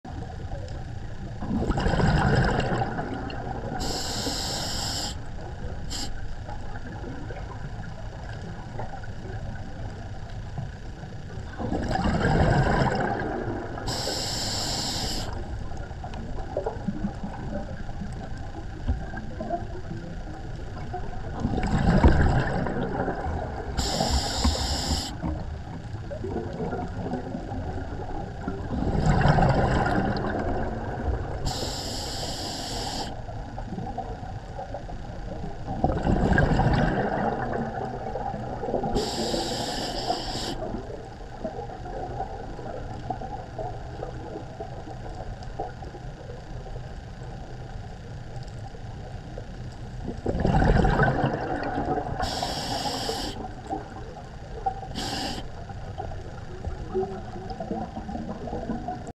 ネコザメ（’２２年１月 静岡県赤沢）
’２２年１月 静岡県 赤沢 ５番
久々になかなかの大きさの ネコザメ さんと遭遇
お顔をもっと撮りたかったのに、落ち葉にサワサワされて嫌がって顔隠しちゃった🤣